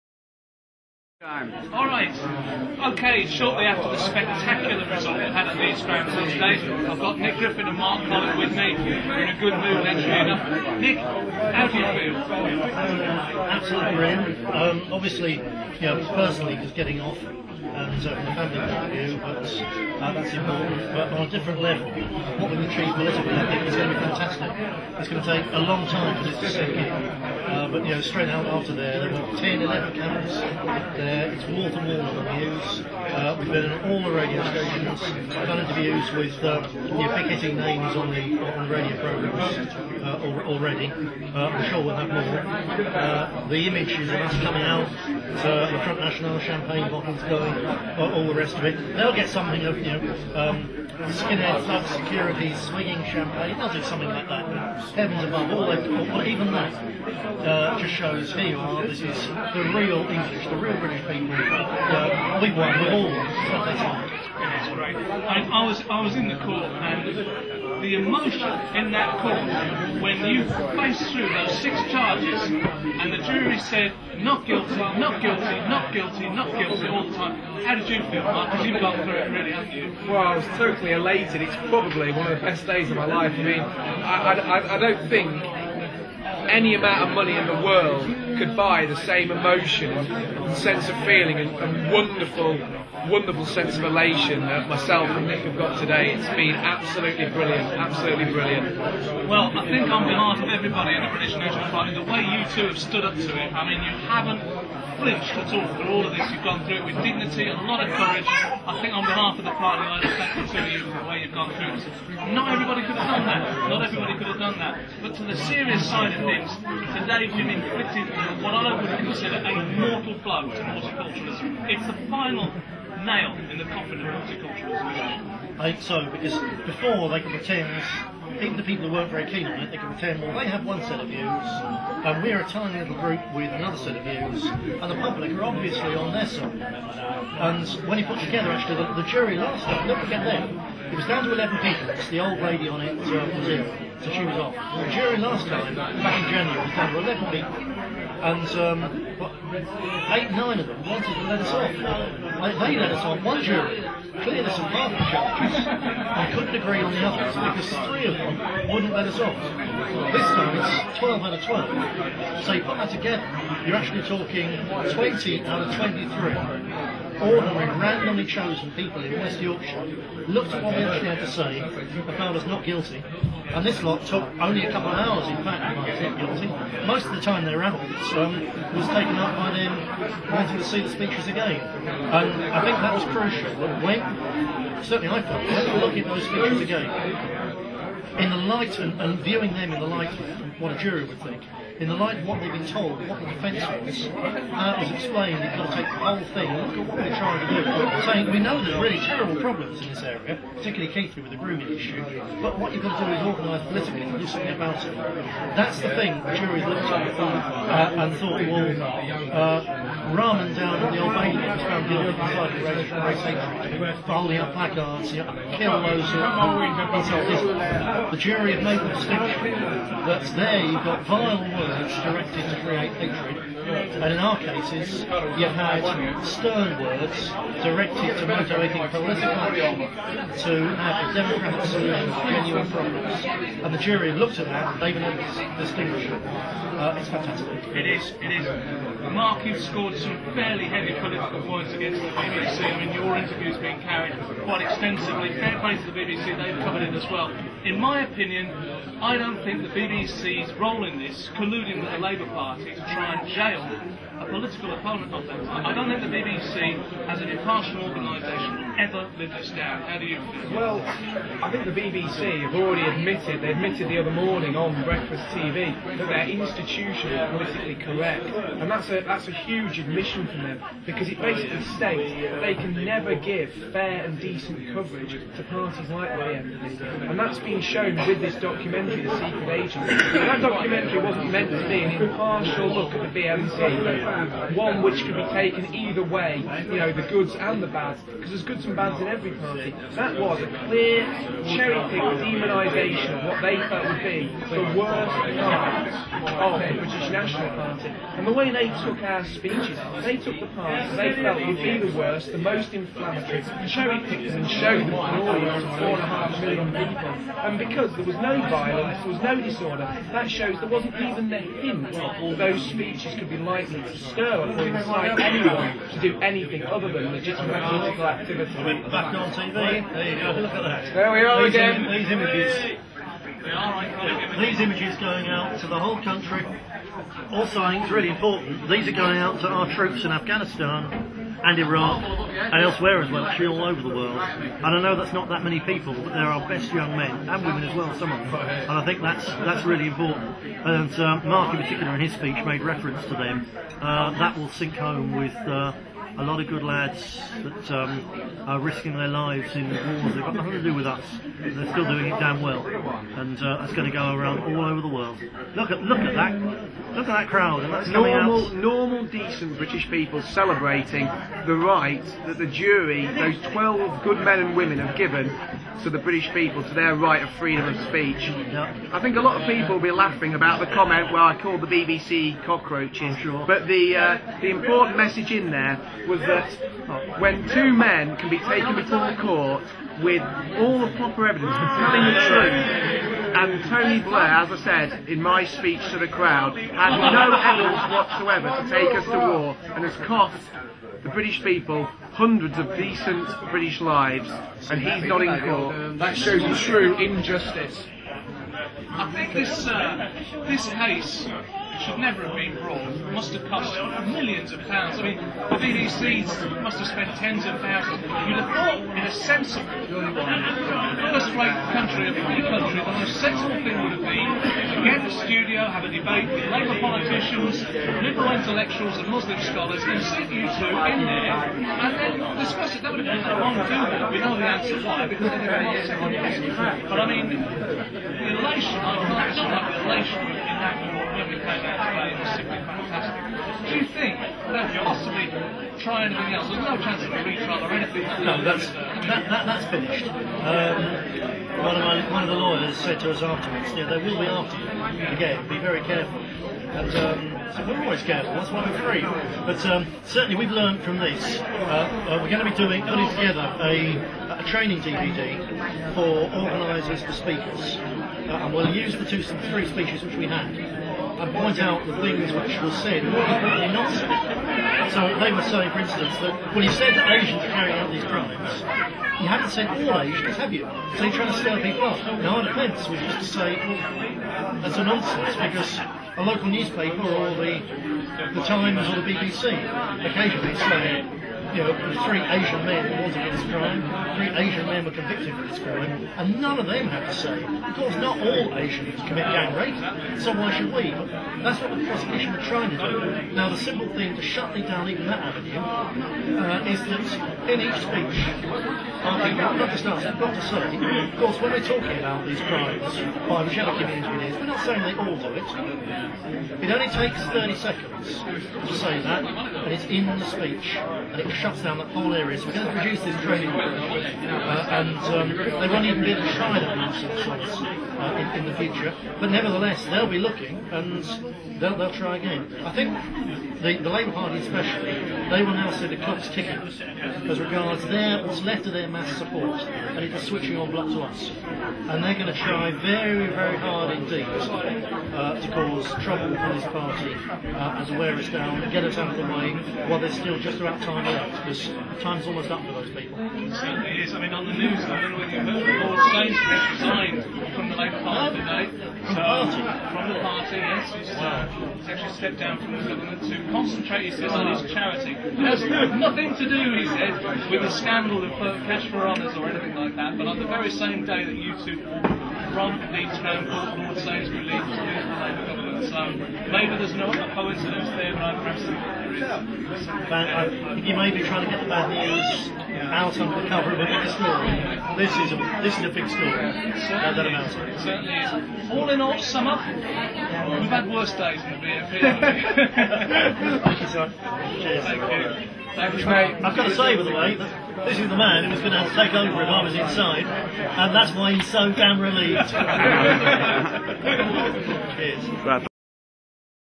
Second Trial: Free Speech - Friday 10th - Reflections
Dated 10-Nov-06.
Simon Darby talks with the Free Speech Two for their comments of the tremendous victory. Against the combined power of New Labour, State broadcaster BBC, West Yorkshire Police and the Crown Prosecution Service this was a victory for common sense, freedom of speech for all political activists, regardless of hue and puts the Labour regime on the back foot.